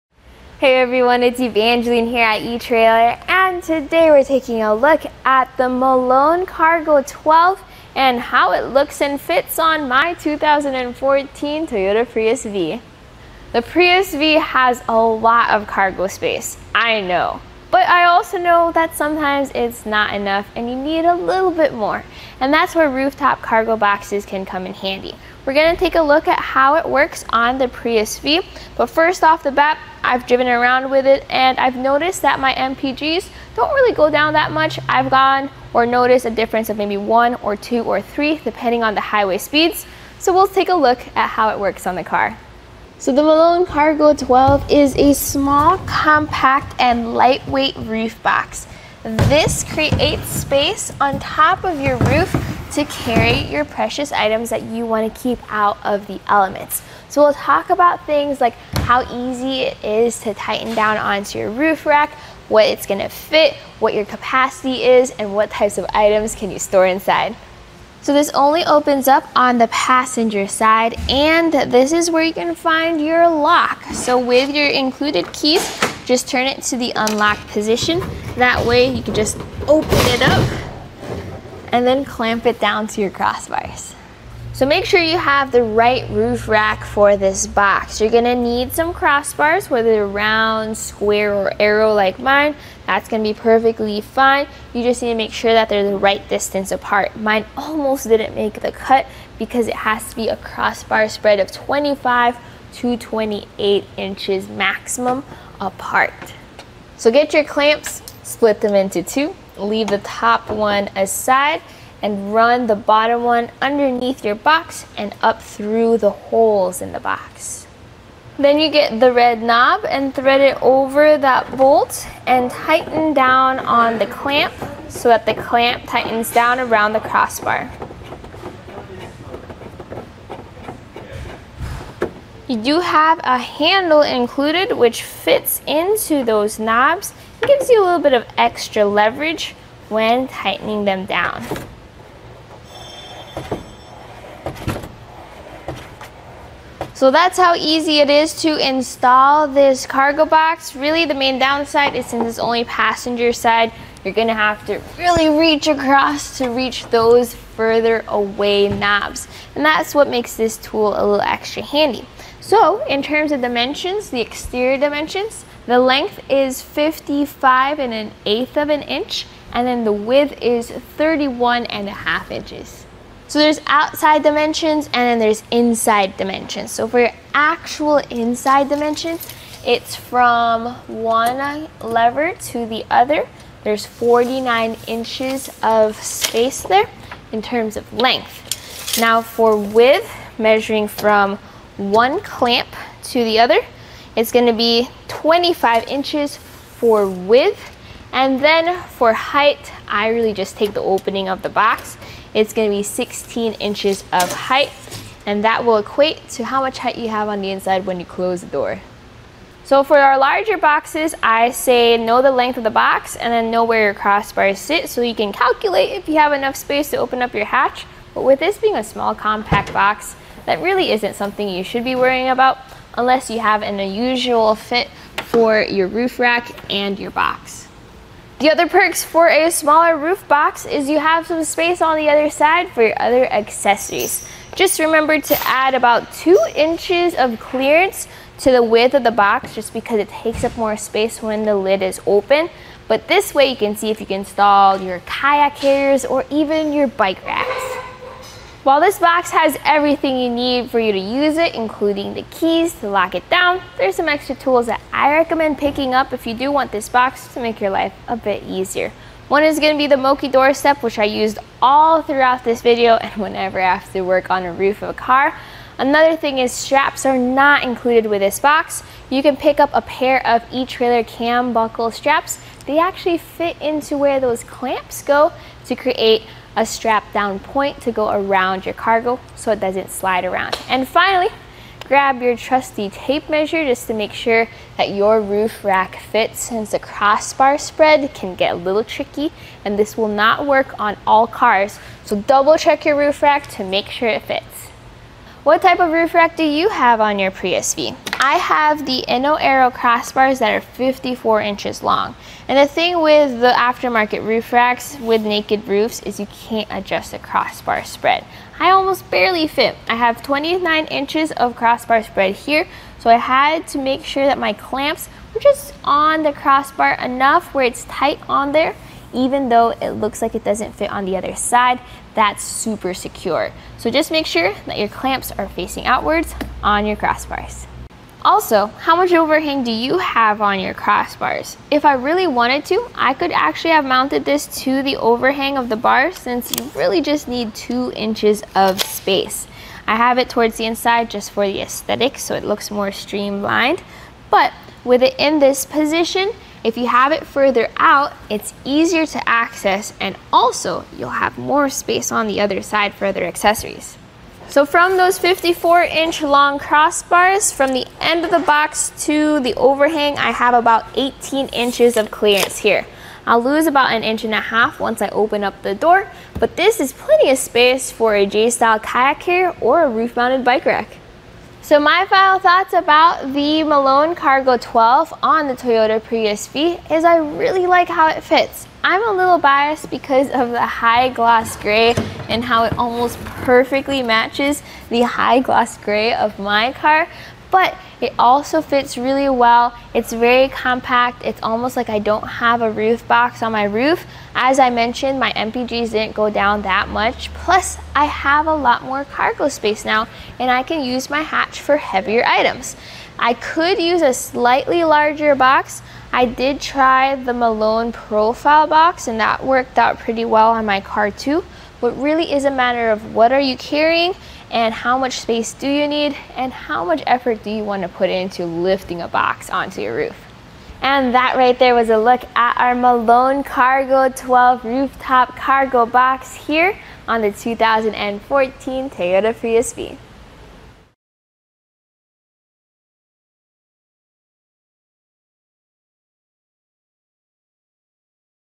etrailer | Setting up the Malone Cargo12 Rooftop Cargo Box on a 2014 Toyota Prius v
Hey everyone, it's Evangeline here at etrailer. And today, we're taking a look at the Malone Cargo 12 and how it looks and fits on my 2014 Toyota Prius V. The Prius V has a lot of cargo space, I know, but I also know that sometimes it's not enough and you need a little bit more. And that's where rooftop cargo boxes can come in handy. We're gonna take a look at how it works on the Prius V. But first off the bat, I've driven around with it and I've noticed that my MPGs don't really go down that much.

I've gone or noticed a difference of maybe one or two or three, depending on the highway speeds. So we'll take a look at how it works on the car. So the Malone Cargo 12 is a small, compact, and lightweight roof box. This creates space on top of your roof to carry your precious items that you wanna keep out of the elements. So we'll talk about things like, how easy it is to tighten down onto your roof rack, what it's gonna fit, what your capacity is, and what types of items can you store inside.

So this only opens up on the passenger side, and this is where you can find your lock. So with your included key, just turn it to the unlocked position. That way you can just open it up and then clamp it down to your crossbars. So make sure you have the right roof rack for this box. You're gonna need some crossbars, whether they're round, square, or arrow like mine that's gonna be perfectly fine.

You just need to make sure that there's a right distance apart. Mine almost didn't make to the cut, because it has to be a crossbar spread of 25 to 28 inches maximum apart. So get your clamps, split them into two, we'll leave the top one aside, and run the bottom one underneath your box and up through the holes in the box. Then you get the red knob and thread it over that bolt and tighten down on the clamp, so that the clamp tightens down around the crossbar. You do have a handle included, which fits into those knobs.

It gives you a little bit of extra leverage when tightening them down. So that's how easy it is to install this cargo box, really the main downside is in this only passenger side, you're gonna have to really reach across to reach those further away knobs. And that's what makes this tool a little extra handy. So in terms of dimensions, the exterior dimensions, the length is 55 1/8 of an inch. And then the width is 31 1/2 inches. So there's outside dimensions and then there's inside dimensions. So for your actual inside dimensions, it's from one lever to the other. There's 49 inches of space there in terms of length. Now for width, measuring from one clamp to the other, it's gonna be 25 inches for width. And then for height, I really just take the opening of the box, it's gonna be 16 inches of height. And that will equate to how much height you have on the inside when you close the door. So for our larger boxes, I say, know the length of the box and then know where your crossbar sits, so you can calculate if you have enough space to open up your hatch. But with this being a small compact box, that really isn't something you should be worrying about unless you have unusual fit for your roof rack and your box. The other perks for a smaller roof box is you have some space on the other side for your other accessories. Just remember to add about two inches of clearance to the width of the box just because it takes up more space when the lid is open. But this way you can see if you can install your kayak carriers or even your bike rack. While this box has everything you need for you to use it including the keys to lock it down, there's some extra tools that I recommend picking up if you do want this box to make your life a bit easier. One is gonna be the multi doorstep, which I used all throughout this video and whenever I have to work on a roof of a car. Another thing is straps are not included with this box. You can pick up a pair of etrailer cam buckle straps. They actually fit into where those clamps go to create a strap down point to go around your cargo so it doesn't slide around. And finally, grab your trusty tape measure just to make sure that your roof rack fits, since the crossbar spread can get a little tricky and this will not work on all cars. So double-check your roof rack to make sure it fits. What type of roof rack do you have on your Prius V I have the Inno Aero crossbars that are are 54 inches long. And the thing with the aftermarket roof racks with naked roof